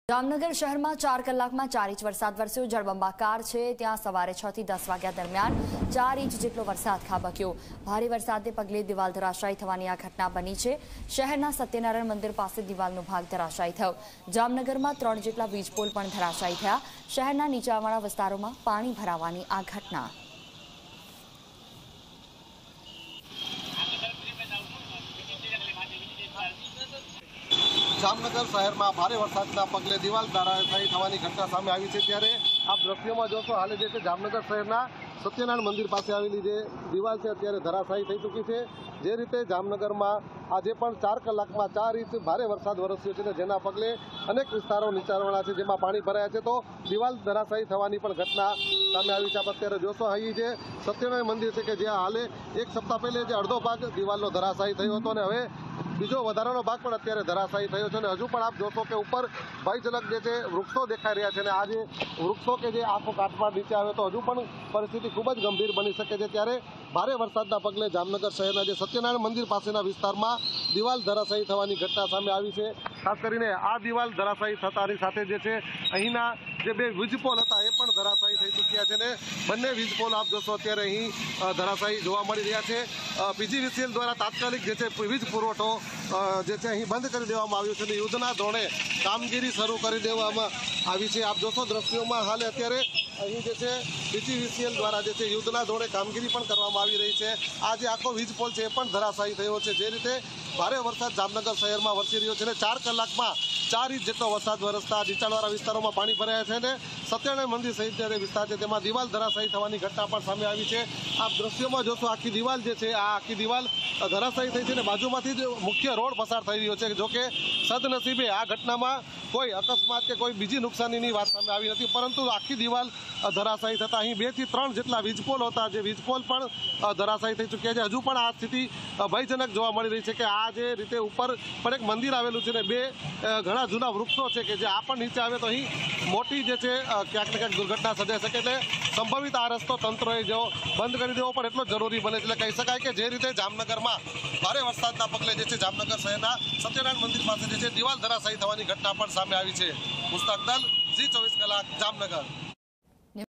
Gujarat Rains: Wall collapses due to heavy downpour in Jamnagar, no casualties reported
ભારે વરસાદના પગલે દિવાલ ધરાશાયી થવાની બની ઘટના --
શહેરના સત્યનારાયણ મંદિર પાસે દિવાલનો ભાગ ધરાશાયી --
જામનગરમાં ત્રણ જેટલા વીજપોલ પણ ધરાશાયી થયા --
શહેરના નીચાણવાળા વિસ્તારમાં પાણી ભરાવાની ઘટના --